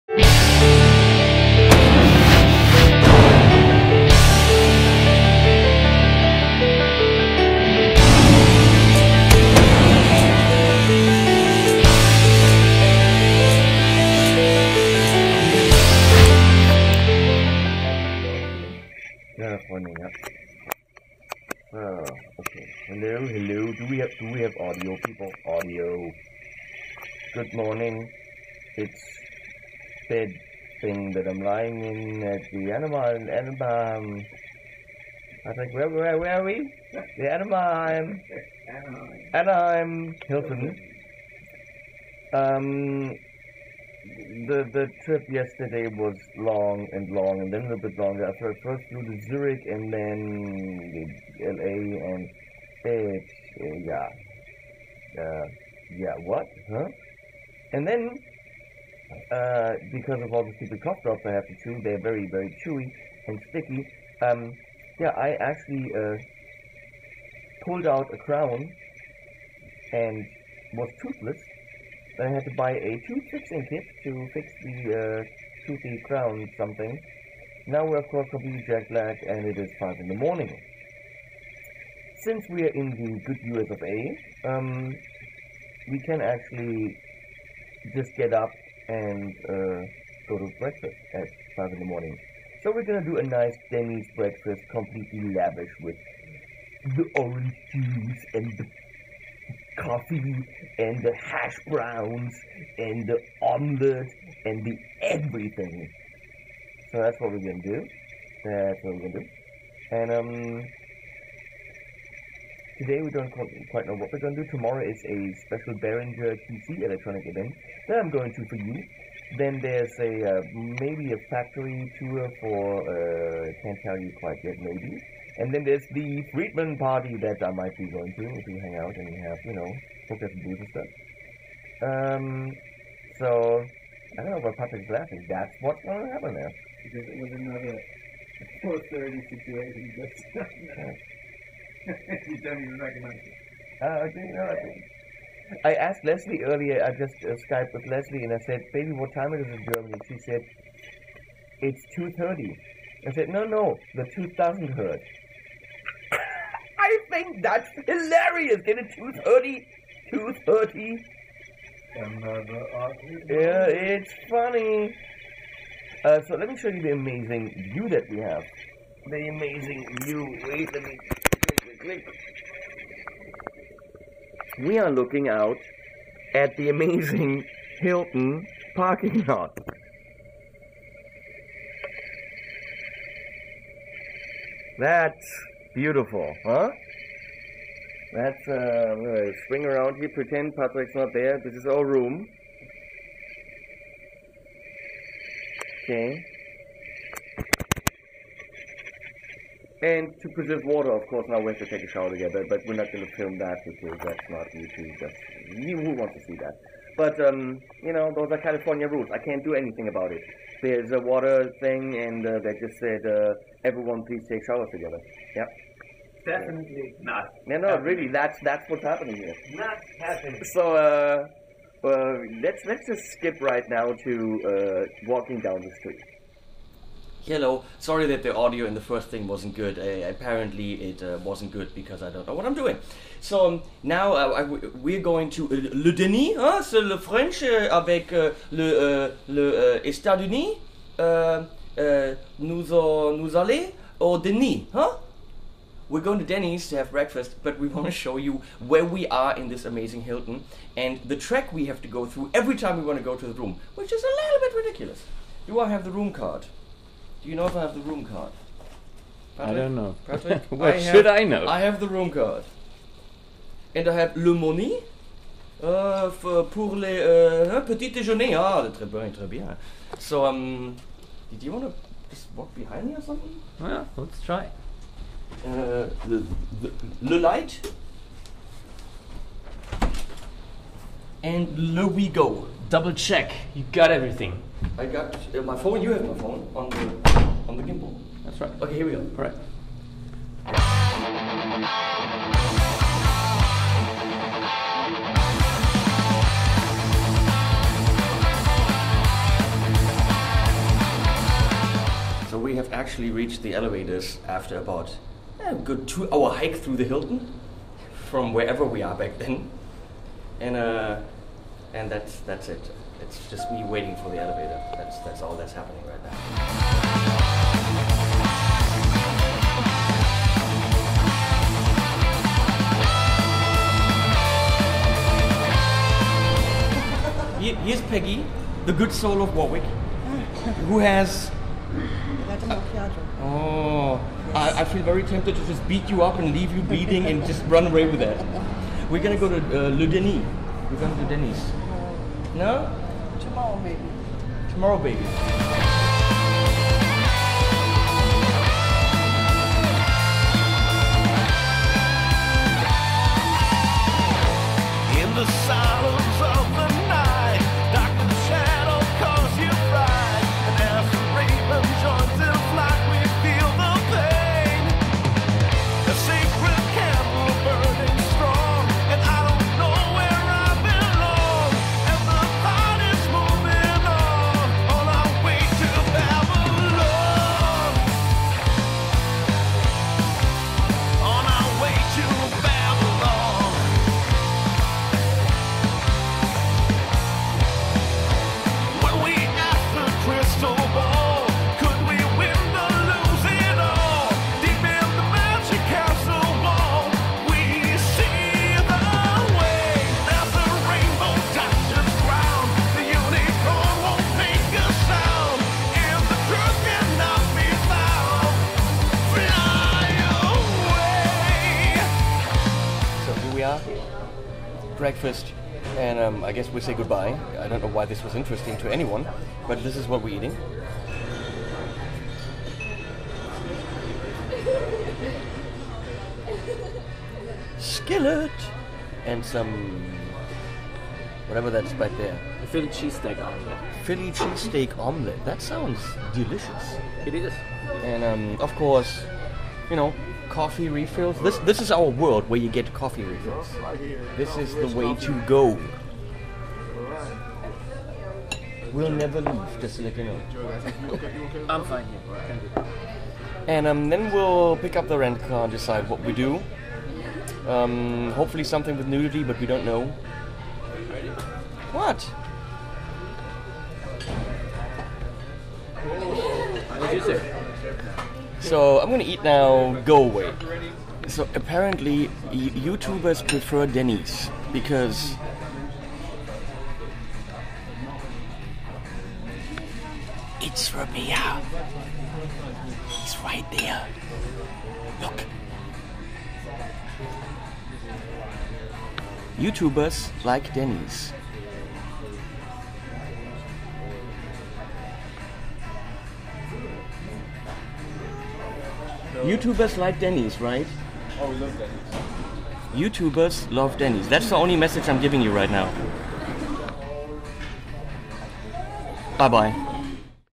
NAMM 2017 - Monday Morning, running into other YouTubers
Jet lag is punishing us. we got up way too early and decided to treat ourselves to a nice Denny's breakfast! Look who we ran into...